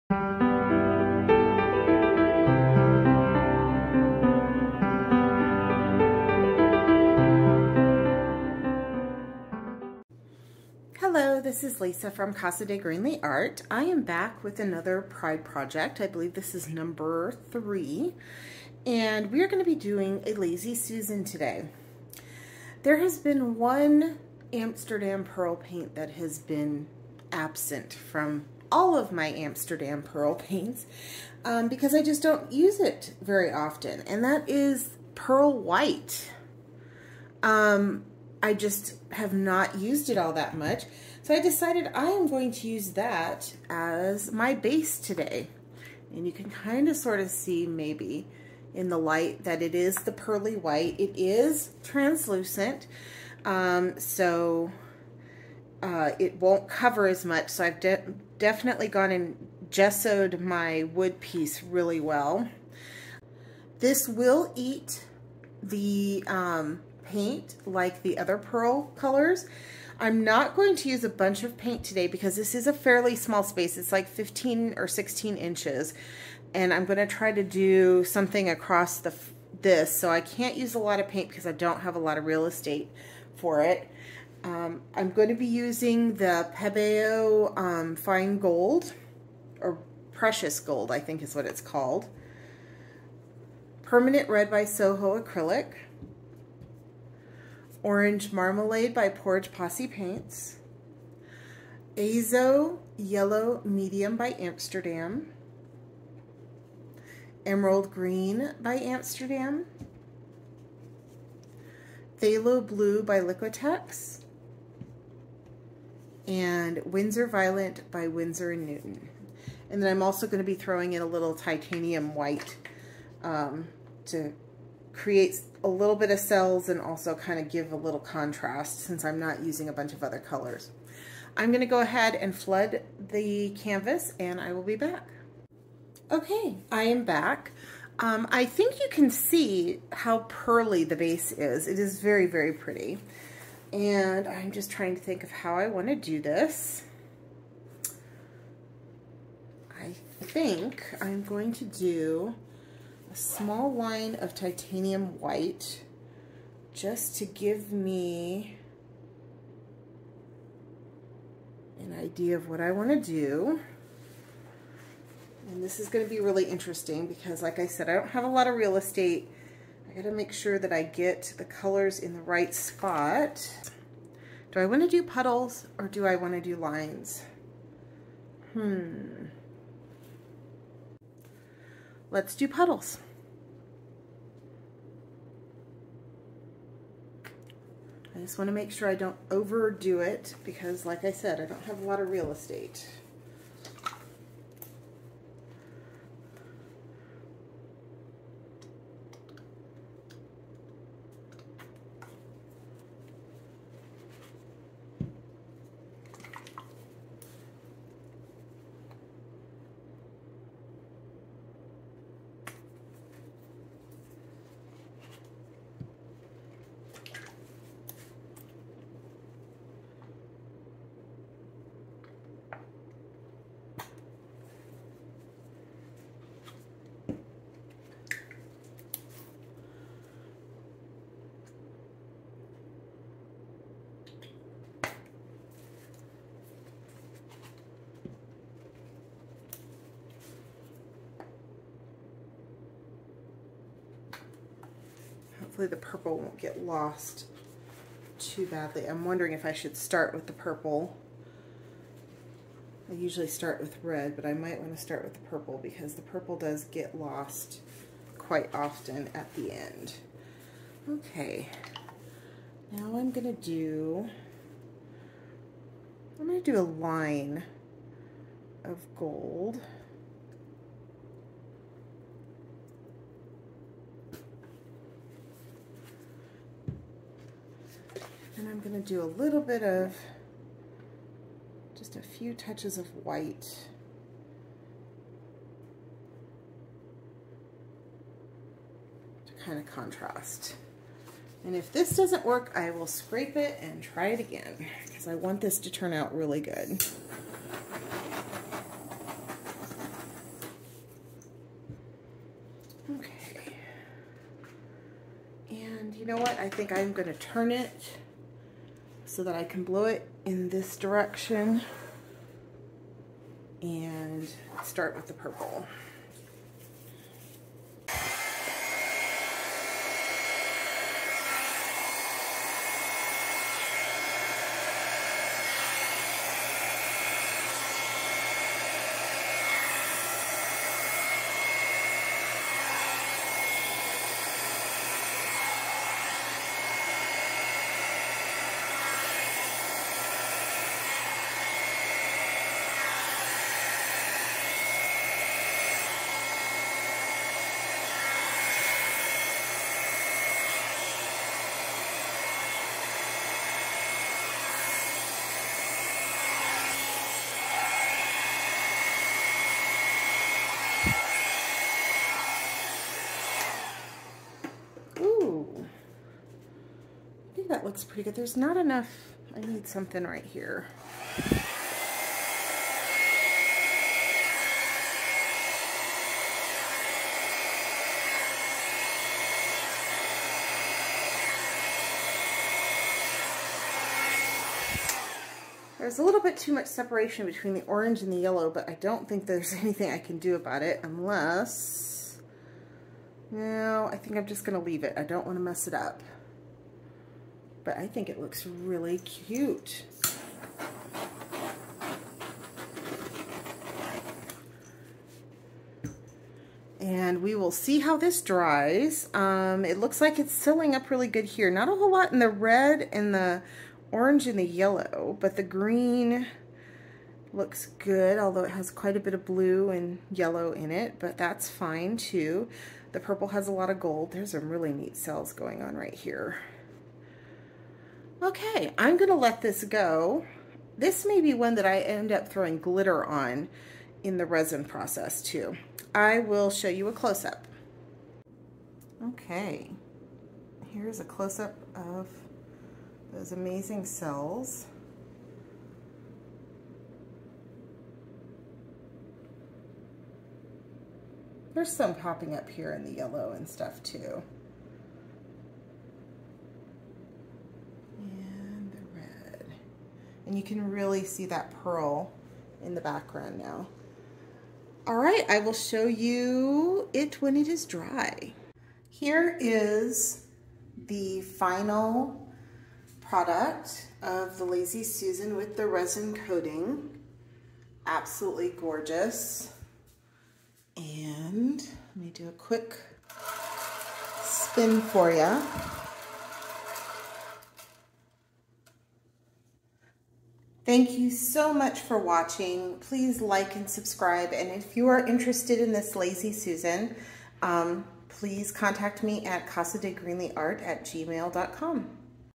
Pride Month Project #4: Lazy-Susan with red-headed step-child of Amsterdam Pearl paints: Pearl White
Paints used:
Pearl White by Amsterdam
Iridescent Precious Gold by Pebeo
Titanium White by Amsterdam
Permanent Red by Soho
Orange Marmalade by Pourage Posse Paints
Azo Yellow Medium by Amsterdam
Emerald Green by Amstrdam
Phthalo Blue by Liquitex
Winsor Violet by Winsor & Newton